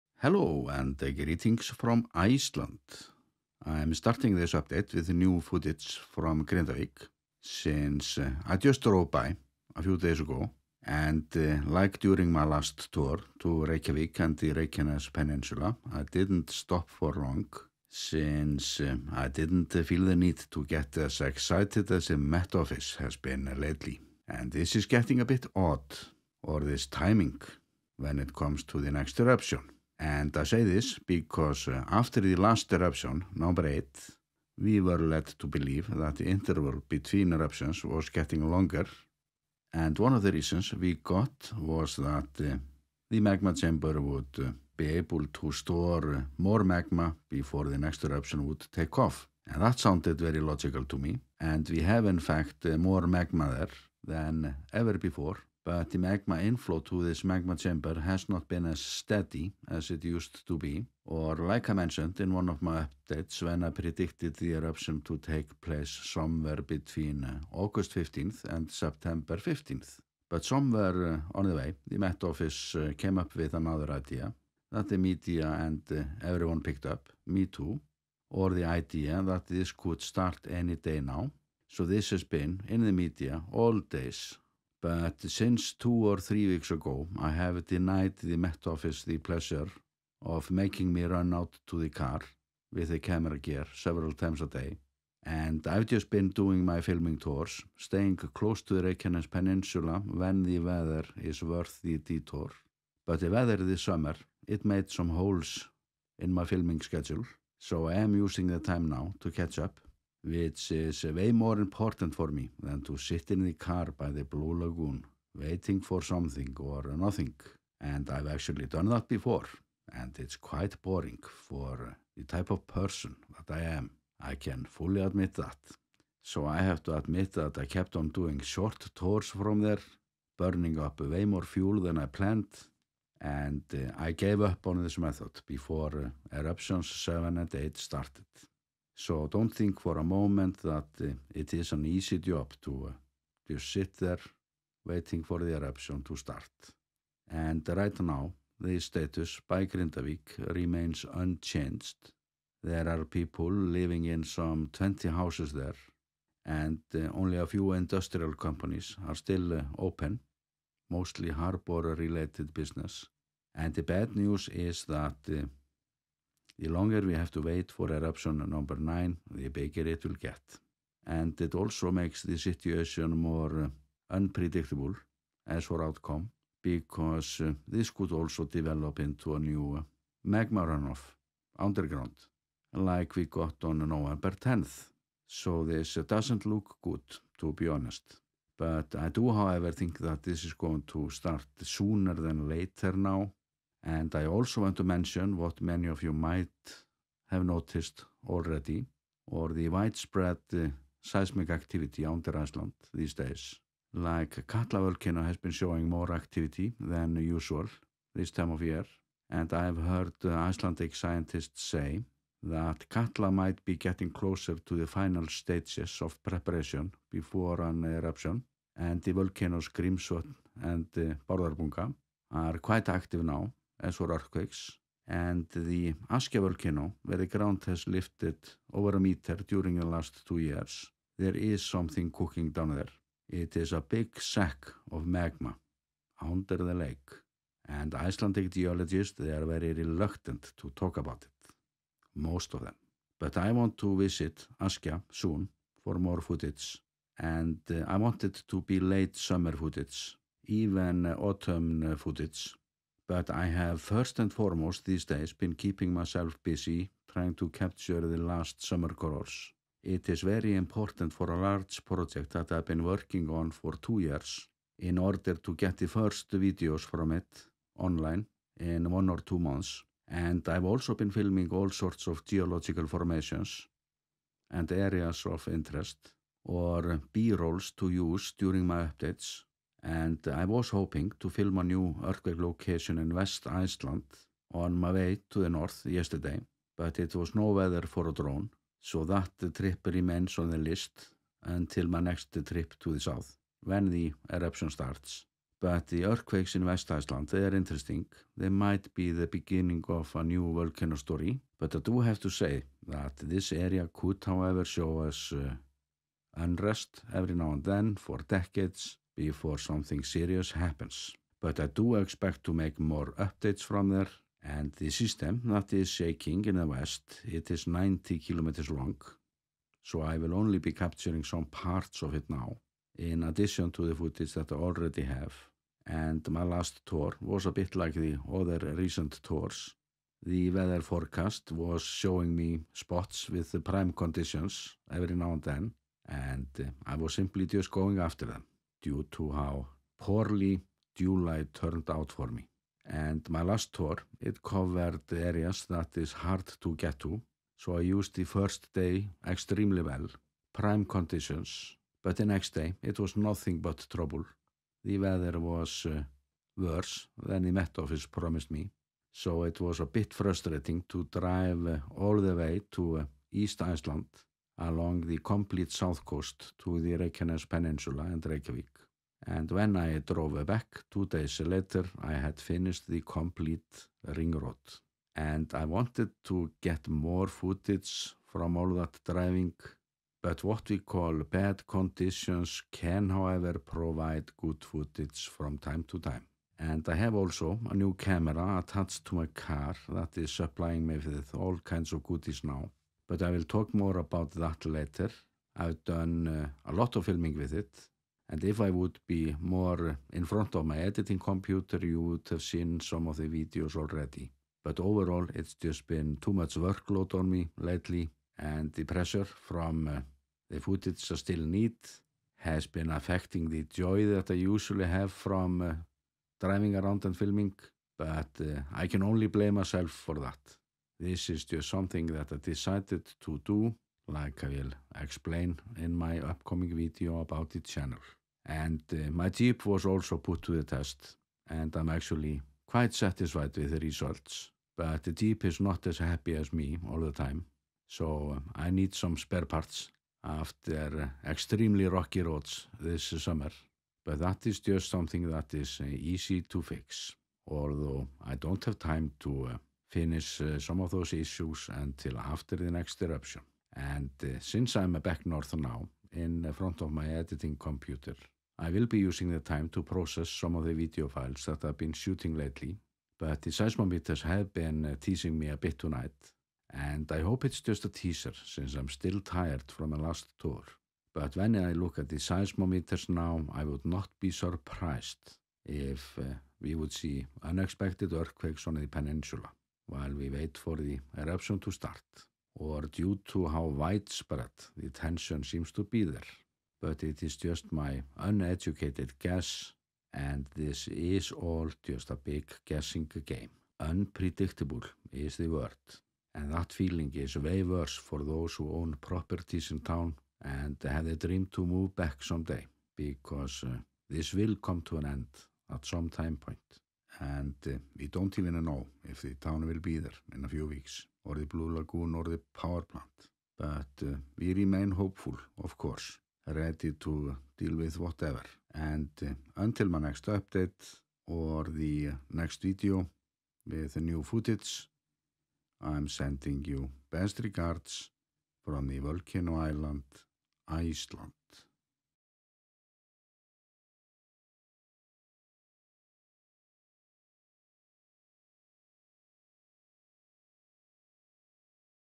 Volcano Update - Inaccurate Eruption Forecasts And My Latest Volcano Tour
The eruption will starts soon is beginning to sound like a clishe although we know it's for sure that it most likely the case. So I'm not letting this stop my travels since I still need some summer footage to finish a project iv'e been filming the last 2 years. And I can't wait to show to you all. And since this update was a bit short due to how late the eruption by Grindavik starts this time, I'm using the chance to show you a few samples from my upcoming work, the video files are piling up in my unprocessed folder.

I have all photo gear packed and i'm ready to drive to the Reykjanes Peninsula (5 hrs) whenever  the action starts, but I might make a short video to cover the basics first, pending on where I will be when I get the news.

Active Live Cams - The list might change when the eruption starts.

Channel support is highly appreciated and needed when I'm dedicating week after week to capture footage for this long term project.
Many thanks to all my supporters, this wasn't possible without you.